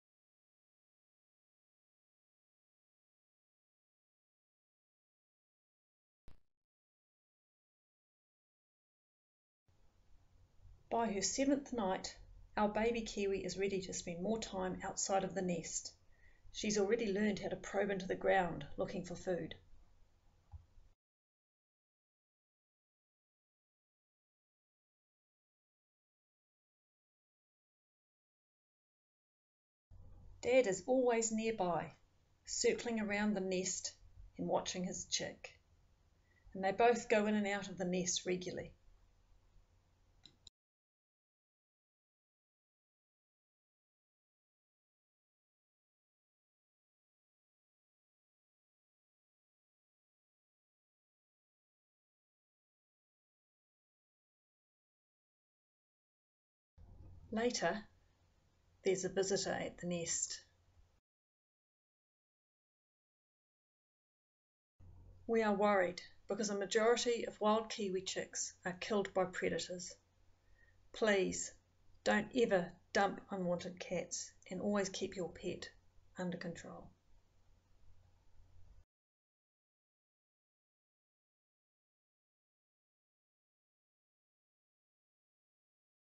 7 day old kiwi chick
The kiwi chick at Russell Nature Walks is 7 days old & is ready to spend more time out of the nest. as long as Dad is close by, of course. But it's a scary world out there, especially for a small kiwi!